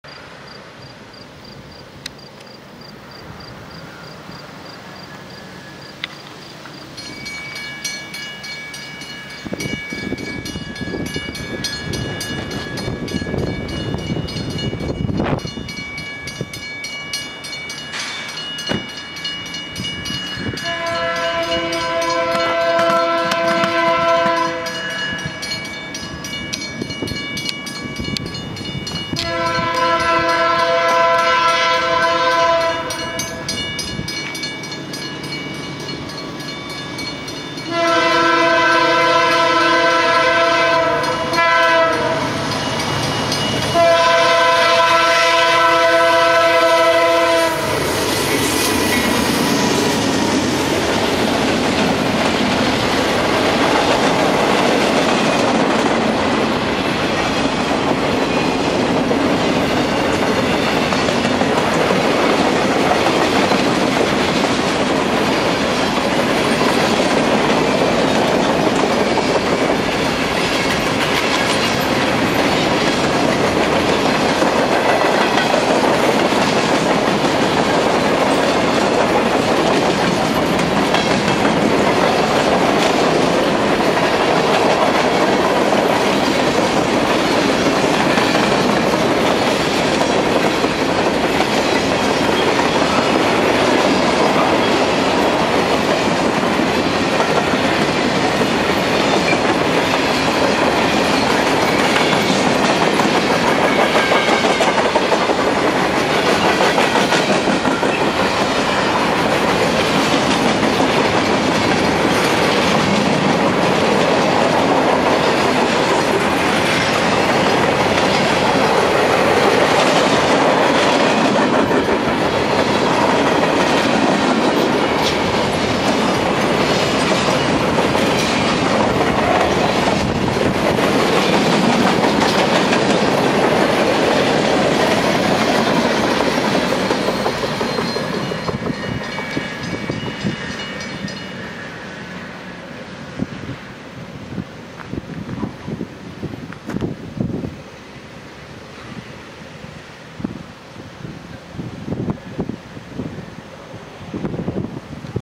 CN 402 Mile 181.24 : CN 2527-5657
Date: August 12 2015
Time: 15h15
MilePost: Mile 181.24 Mont-Joli Subdivision

Power:
CN 2527 (Ge Dash9-44cwl)
CN 5657 (Emd Sd75i)

Number of cars:xx cars